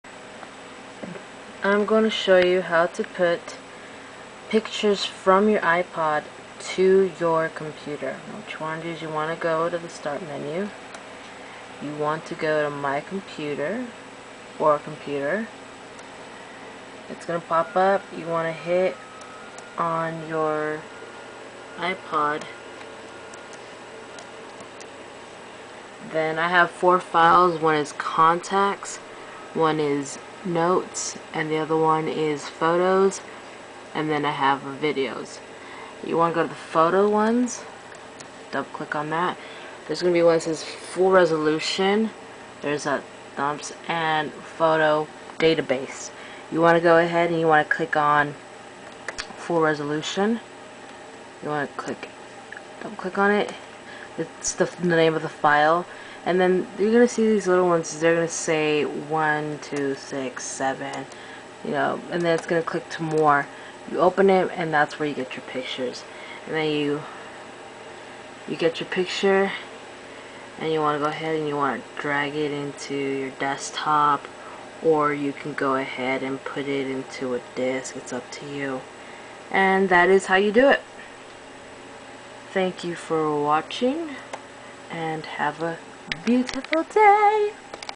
how to transfer pictures from your ipod to your computer
this video will hopefully show you how to transfer photos from your ipod to your computer